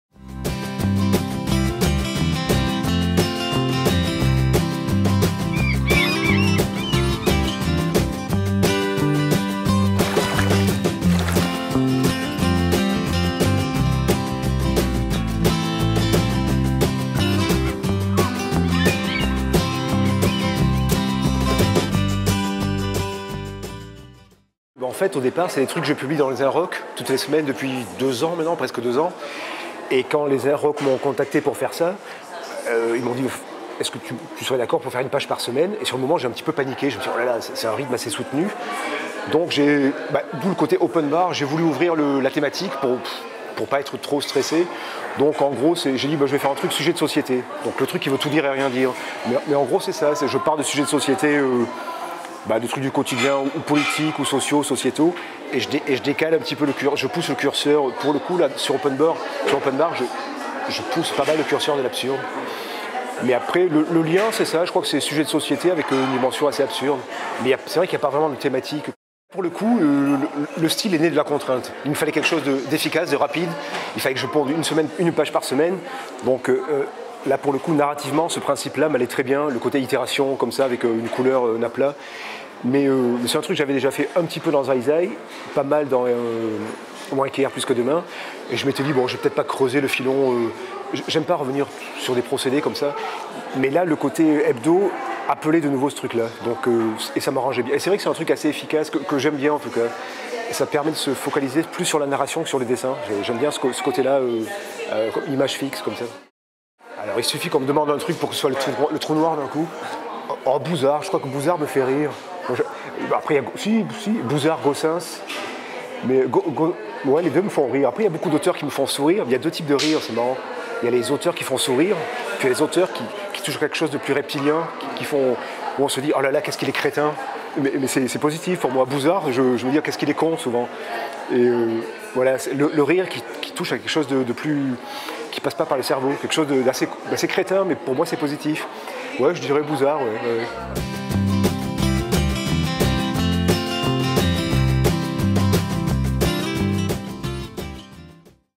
Fabcaro : Open Bar, 1ère tournée
À l'occasion du festival 2019 "Oh les beaux jours !" de Marseille, rencontre avec Fabcaro (Fabrice Caro) autour de son ouvrage  "Open Bar : 1ère tournée" aux éditions Delcourt.